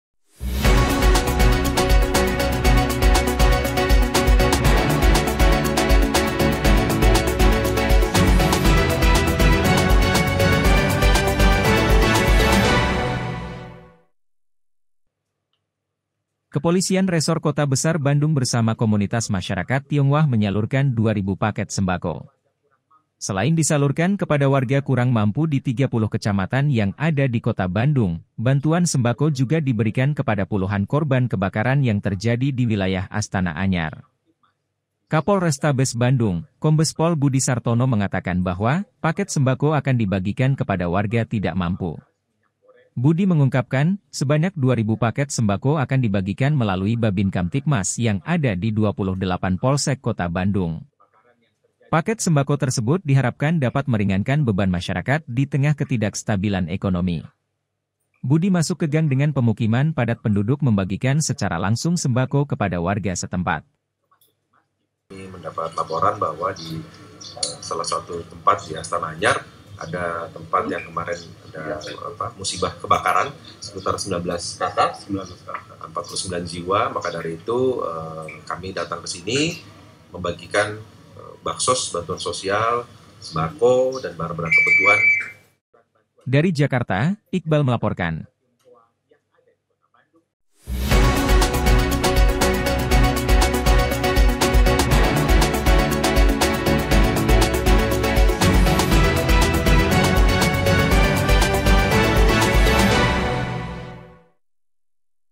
POLRESTABES BANDUNG BERSAMA KOMUNITAS MAMPU BERIKAN PAKET SEMBAKO
Kepolisian Resor Kota Besar Bandung bersama Komunitas Masyarakat Tionghoa menyalurkan 2.000 paket sembako. Selain disalurkan kepada warga kurang mampu di 30 kecamatan yang ada di Kota Bandung, bantuan sembako juga diberikan kepada puluhan korban kebakaran yang terjadi di wilayah Astana Anyar.

Kapolrestabes Bandung, Kombes Pol Budi Sartono mengatakan bahwa, paket sembako akan dibagikan kepada warga tidak mampu. Budi mengungkapkan, sebanyak 2000 paket sembako akan dibagikan melalui Bhabinkamtibmas yang ada di 28 Polsek Kota Bandung. Paket sembako tersebut diharapkan dapat meringankan beban masyarakat di tengah ketidakstabilan ekonomi.

Budi masuk ke gang dengan pemukiman padat penduduk membagikan secara langsung sembako kepada warga setempat.

EDITOR : IQBAL